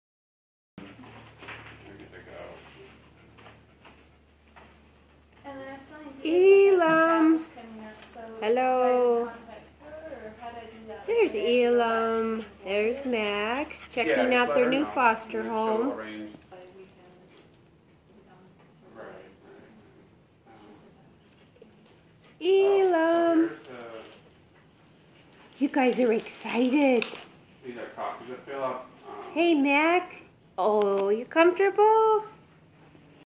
Lucky Dogs, Maxx and Elom
Adoptable long haired Dachshunds available with Lucky Dog Rescue in Scottsdale, AZ.